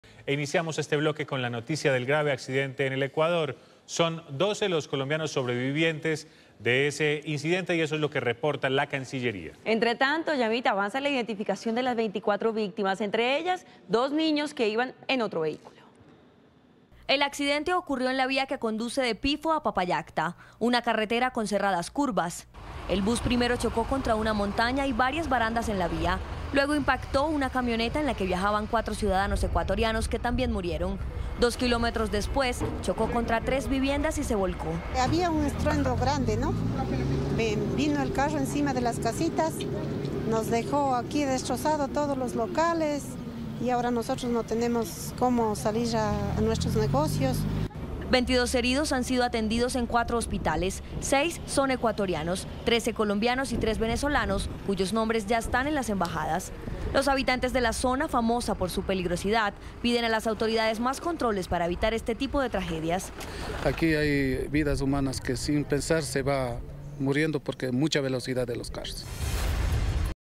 Ecuador destituye a funcionarios de tránsito por accidente que dejó 24 muertos
El presidente Lenín Moreno separó de sus cargos a varios directivos, entre ellos el director de la Policía de Tránsito, por los errores detectados en el control del ingreso del bus al país.